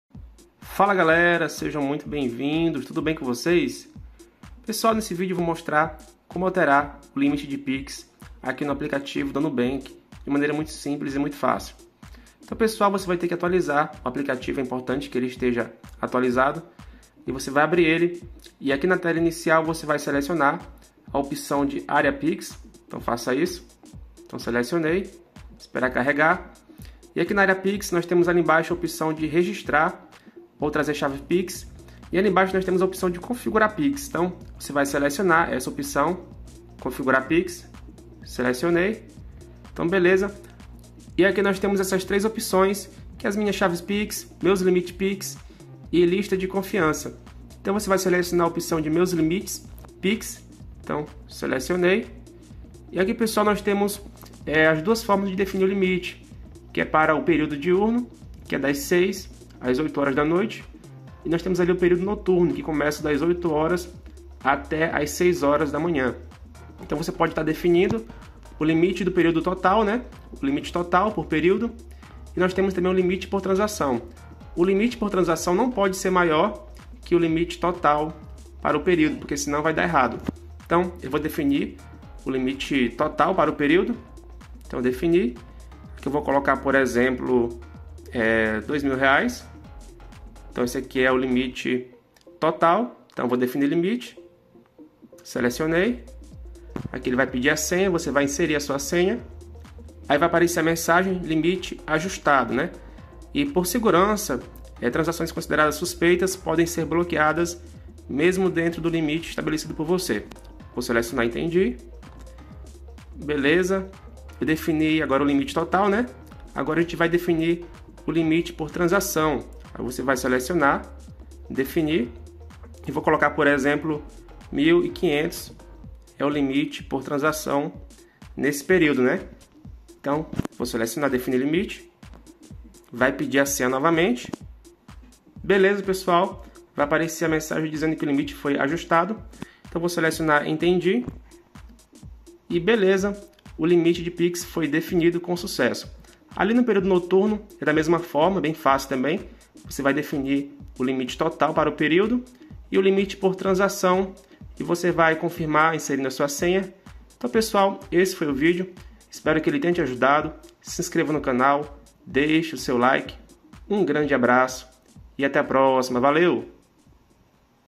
Como Alterar Limite de Pix Nubank (Rápido e Fácil)
Nesse vídeo mostro passo a passo como alterar o limite de Pix no App do Nubank, falo também sobre como funciona o limite por transação e o limite total e como você deve alterar eles.